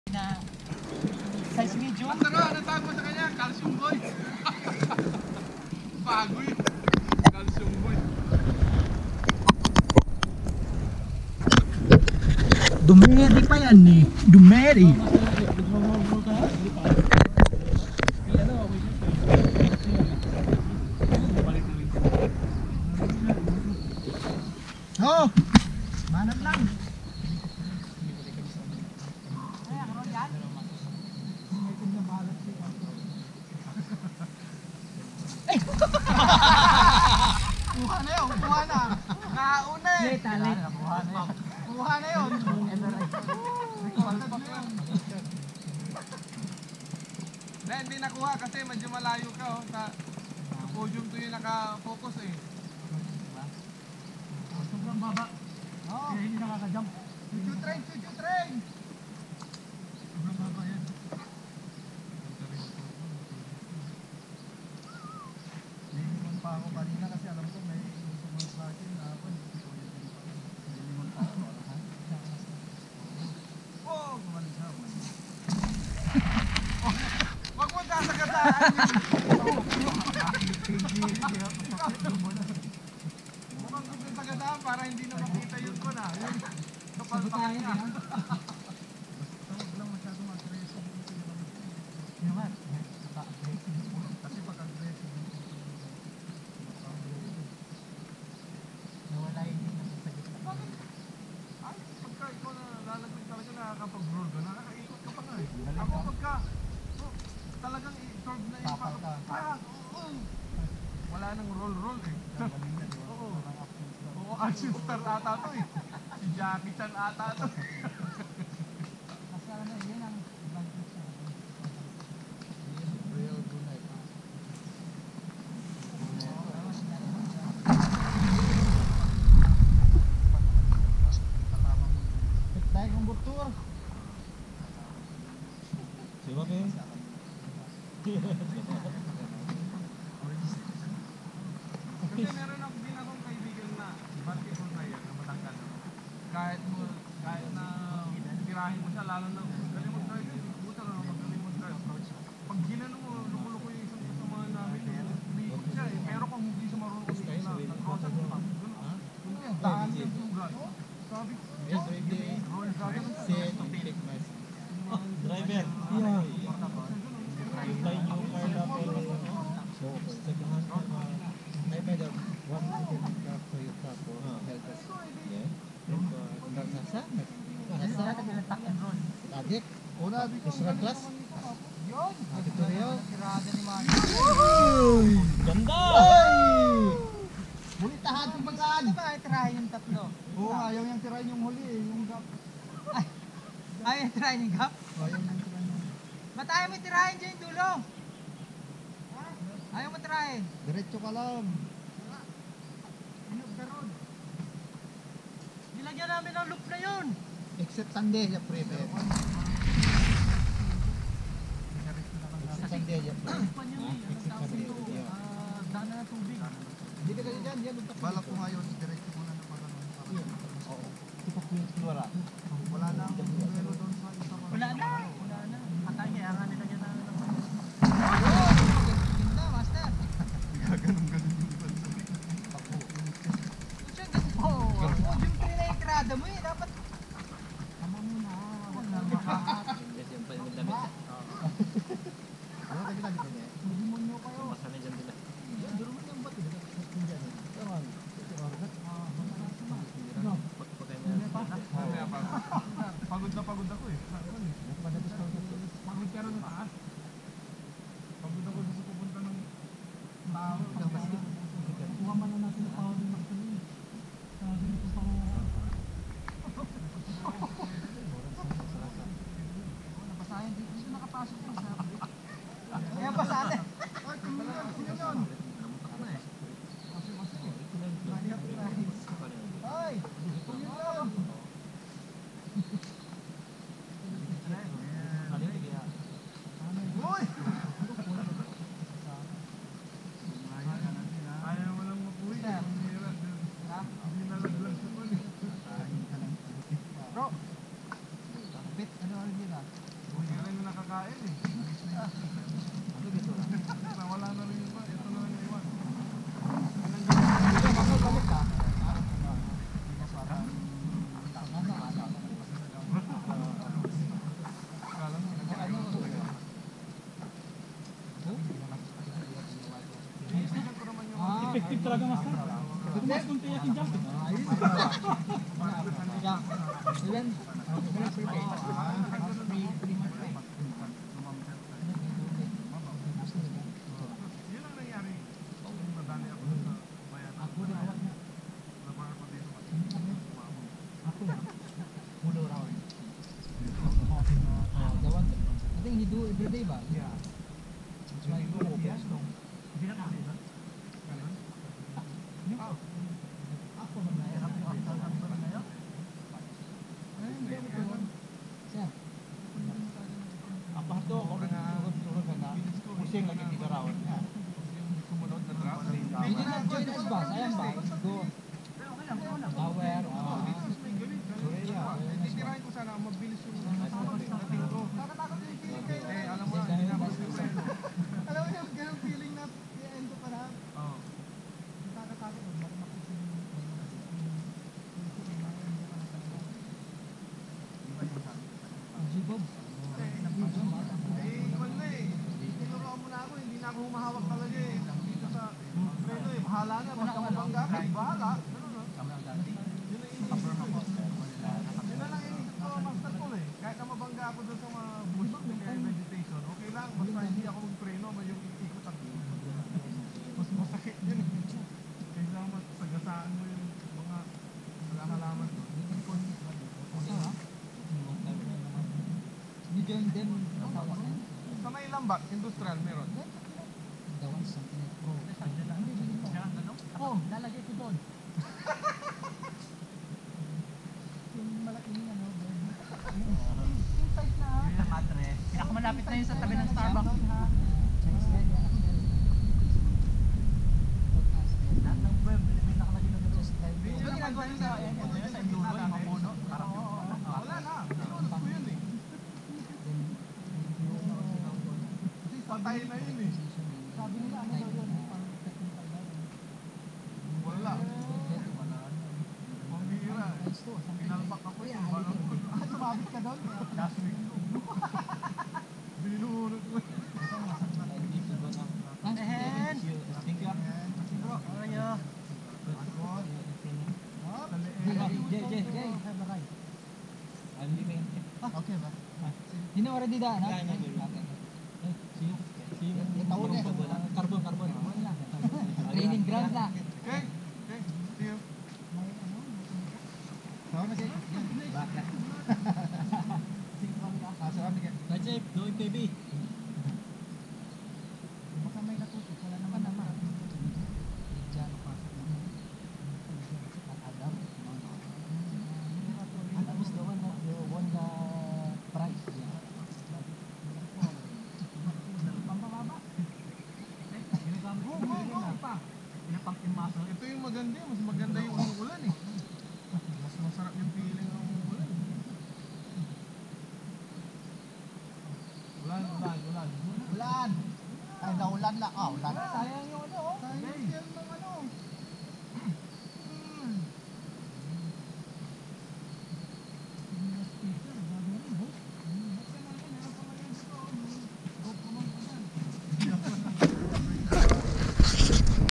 Raw Video #241 - Pinoy Podium Jumps - Chillin' Practice 01
Pinoy MTBikers Brunei taking their rest at Pinoy Podium Jumps while others having practice on the Jumps